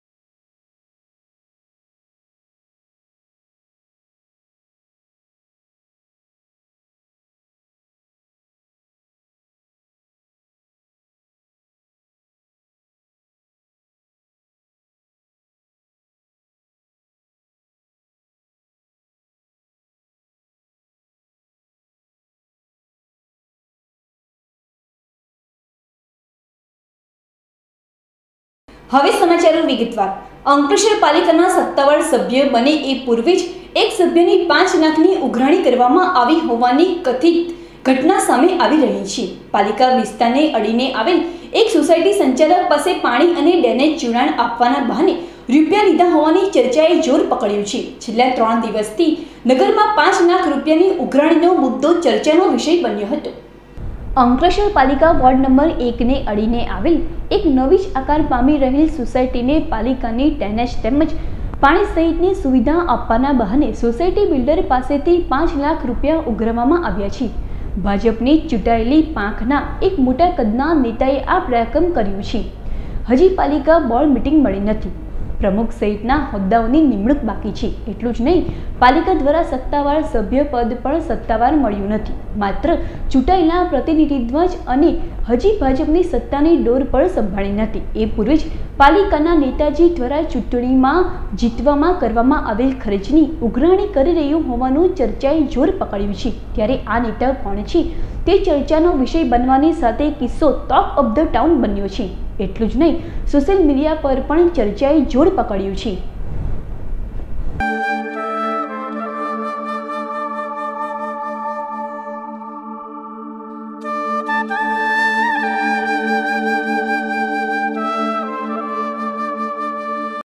માં ટીવી 16.03.2021
અંકલેશ્વર પાલિકાના સત્તાવાર સભ્ય બને એ પૂર્વે જ એક સભ્યની 5 લાખની ઉઘરાણી
પાલિકા વિસ્તાર ને અડીએ આવેલ એક સોસાયટી સંચાલક પાસે પાણી અને ડ્રેનેજ જોડાણ આપવાના બહાને રૂપિયા લીધા હોવાની ચર્ચા : છેલ્લા 3 દિવસ થી નગરમાં 5 લાખ રૂપિયાની ઉઘરાણીનો મુદ્દો ચર્ચાનો વિષય બન્યો : પાલિકાની ચૂંટણીમાં કરેલ ખર્ચની વસુલાતની શરૂઆત કરાય હોવાની ચર્ચા